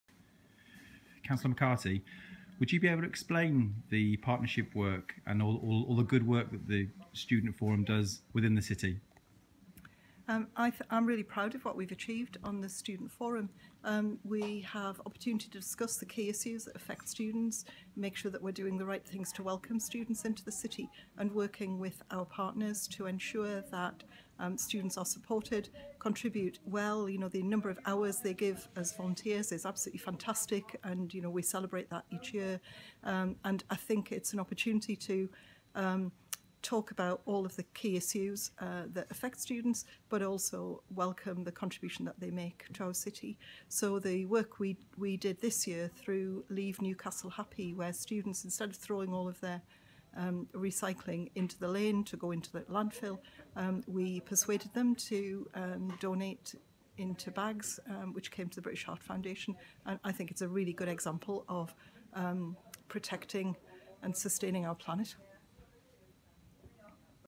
British Heart Foundation collection campaign
Cllr Joyce McCarty, Deputy Leader of Newcastle City Council and Chair of the Student Forum

The British Heart Foundation (BHF) shops’ partnership with Newcastle University, NUSU, Northumbria University and Newcastle City Council which encouraged students to donate their unwanted items at the end of term, has proved to be a huge success and could have raised up to £87,220 for the BHF. The profits raised will go into the fight against heart disease and help fund the BHF’s life-saving research.